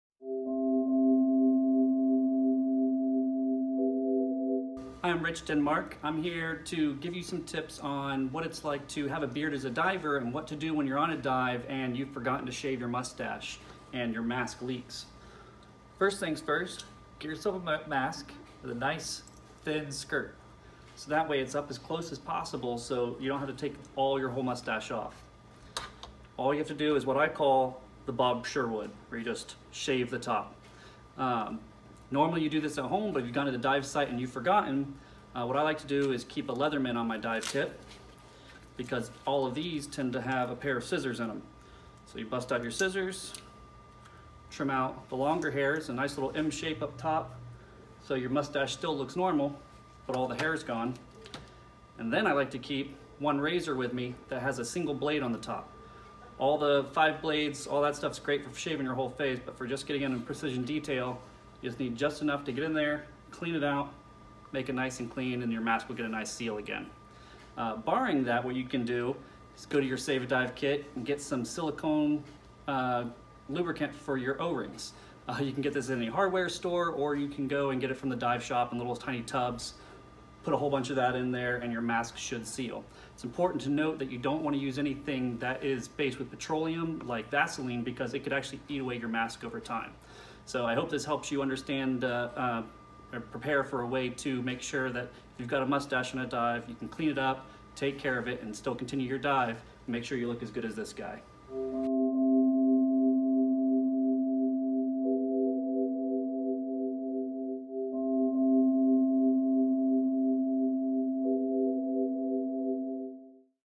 Diving Tips and Tricks: Save a Dive Beard Kit
No one wants to call a dive because their mask keeps leaking, nor do you want to shave off that beard you have worked so hard on. GUE’s Rich Denmark’s on the job with some face saving tips. Got your “Save-a-Beard” kit handy?